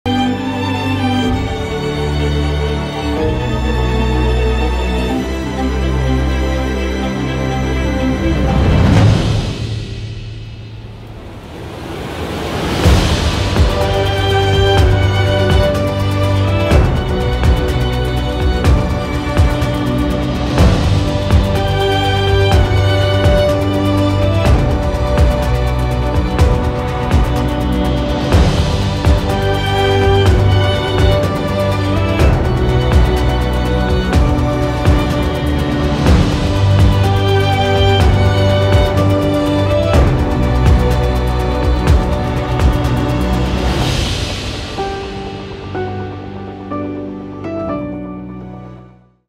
The Dubai Balloon - Rise Above The Everyday
Feel on top of the world and experience wonder at adrenaline-rushing heights of up to a spectacular 300 meters, or a 100-storey building. Marvel at the stunning shape of Palm Jumeirah and take in incredible views of Dubai and its glistening skyline with an experience that offers more than just an observation point.